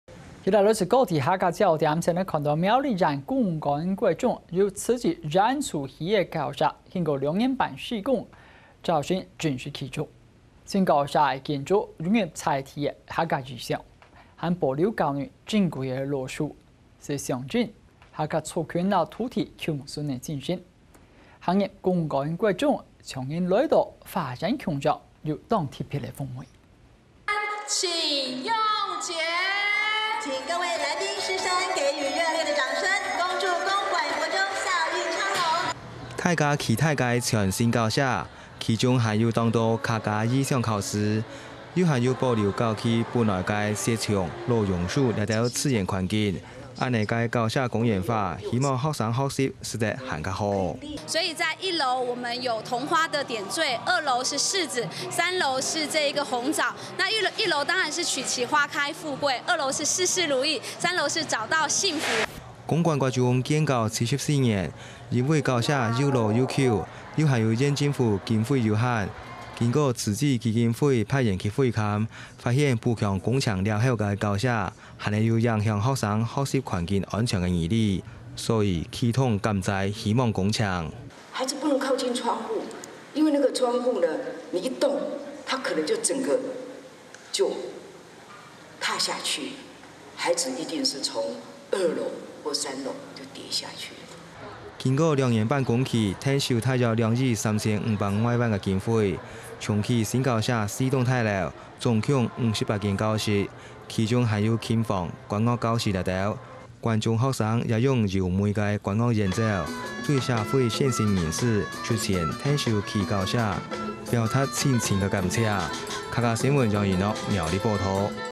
保留老樹.校園公園化 苗公館國中新校舍啟用【客家新聞20201020】
苗栗縣公館國中慈濟援建校舍，經過2年半施工，20日正式啟用，新校舍建築融入在地客家意象，並保留校內珍貴老樹，象徵客家族群與土地共存精神，走進公館國中，彷彿置身花園一般，別有一番風味。